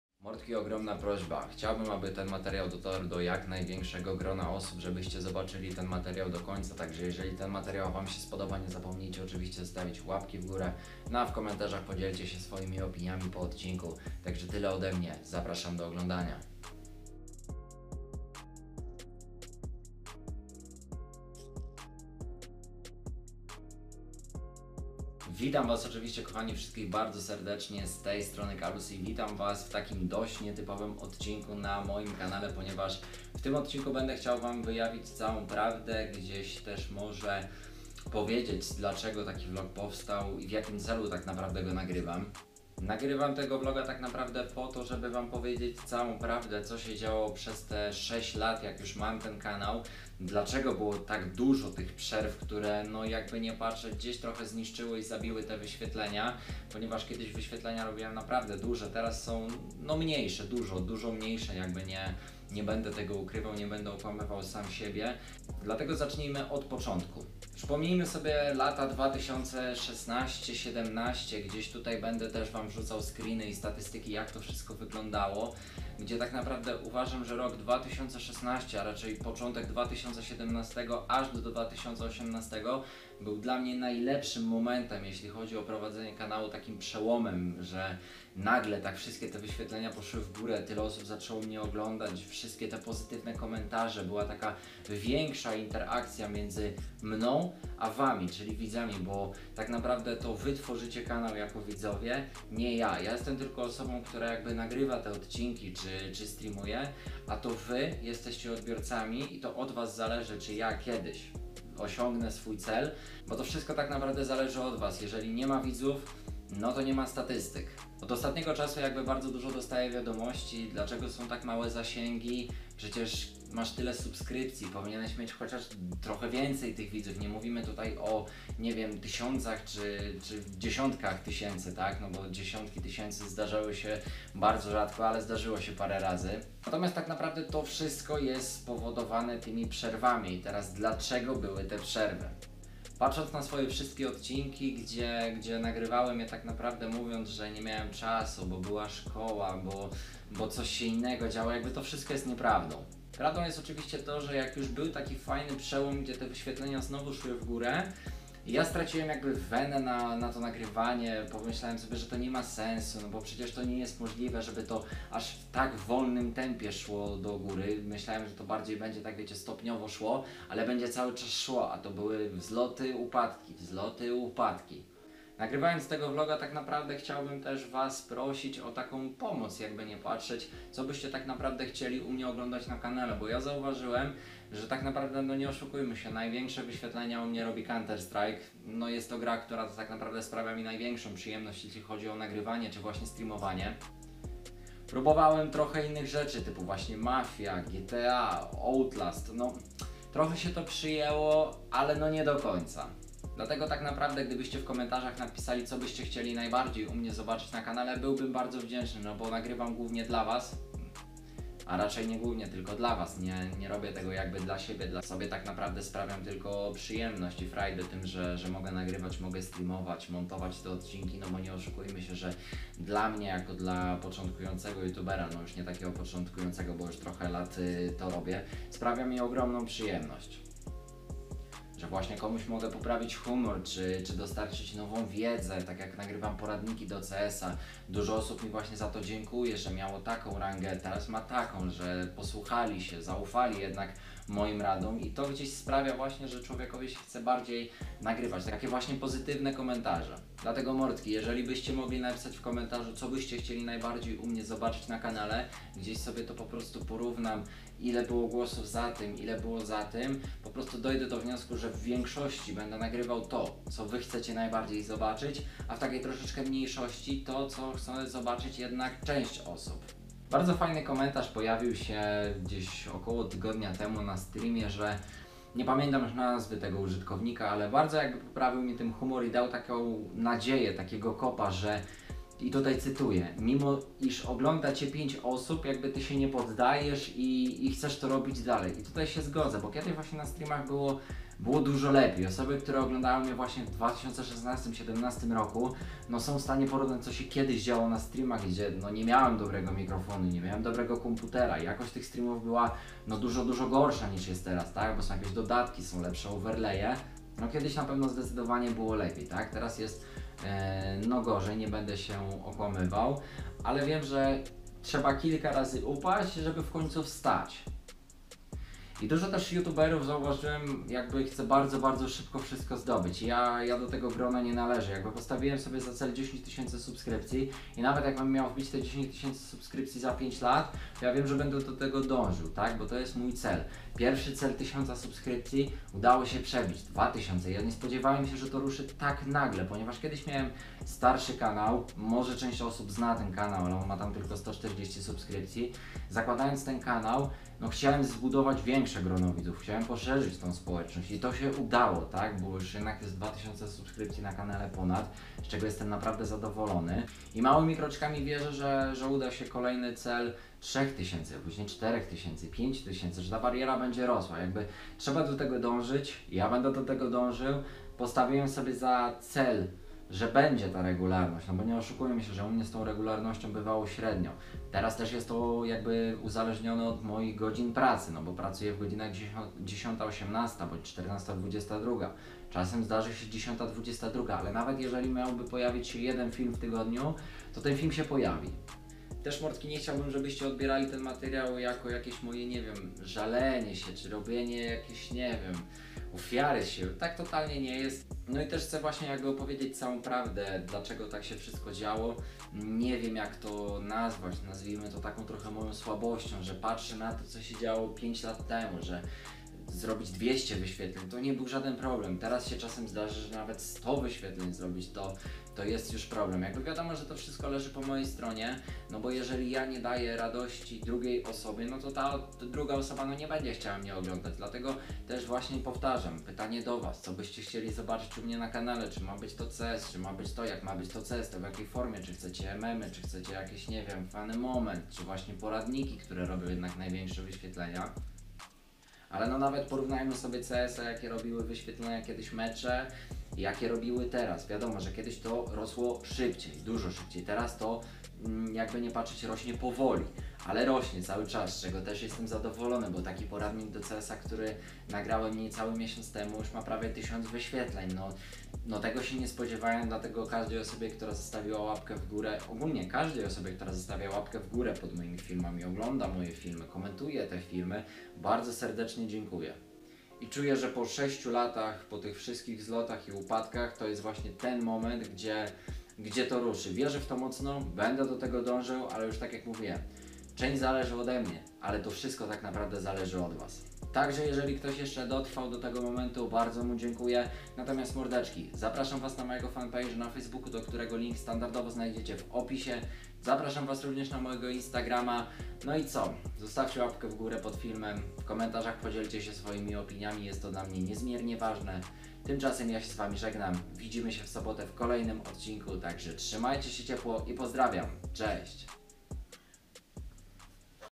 POROZMAWIAJMY CZYLI CAŁA PRAWDA WRAZ Z PROŚBĄ DO WIDZÓW [KalusTV vlog]
POROZMAWIAJMY CZYLI CAŁA PRAWDA WRAZ Z PROŚBĄ DO WIDZÓW
Mordki dzisiejszy vlog jest bardzo ważny dla mnie chciałbym wyznać wam całą prawdę jak to wszystko wyglądało na przestrzeni 5 lat od momentu założenia kanału.
W komentarzach podzielcie się swoimi opiniami co można dodać poprawić itp
Oraz co byście chcieli najbardziej zobaczyć na kanale z góry dziękuje :)

👍Zostaw like dla zasięgu!

🔴Strzel suba na kanale wraz z dzwoneczkiem a niczego nie przegapisz !

💬💬Zostaw komentarz a przypnę ci ❤💬💬💬